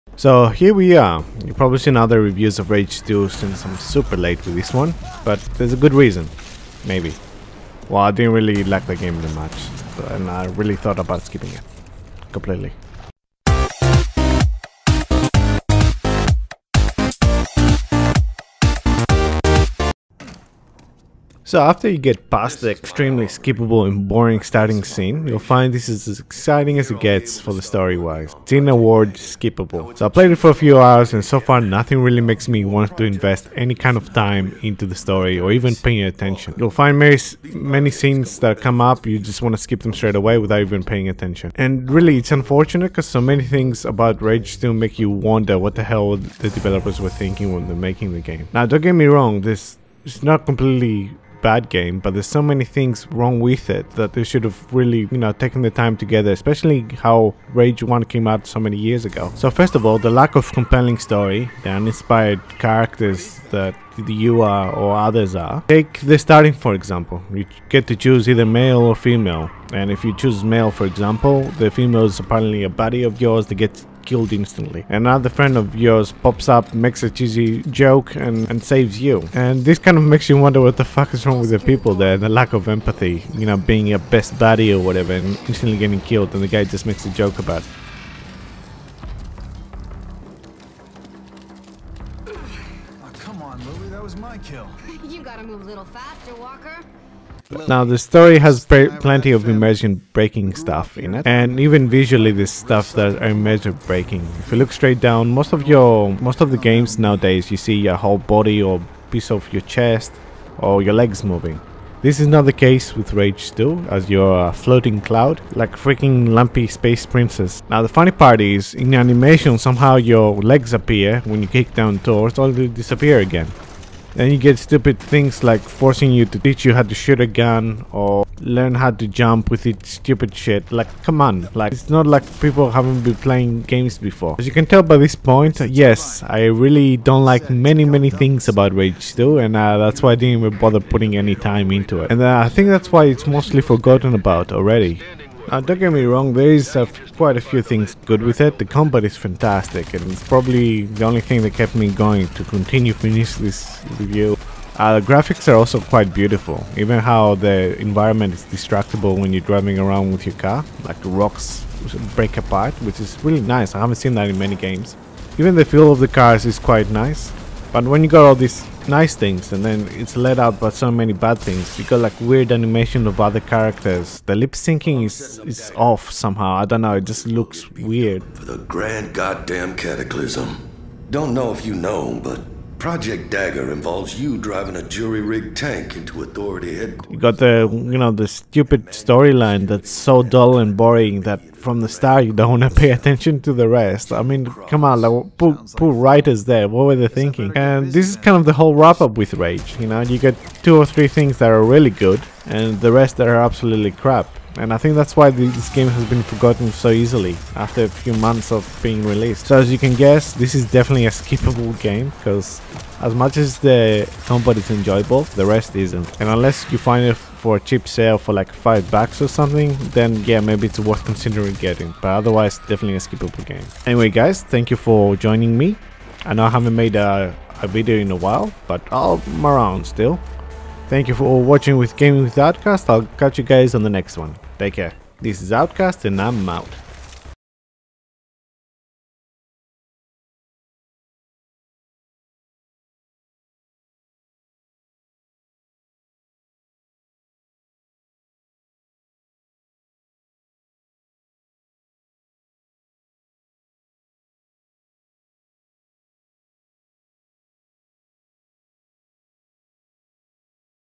Rage 2 review - Why it's forgotten and not missed
Here's my review and thoughts on Rage 2 from Bethesda games.
Sorry late but better late than never.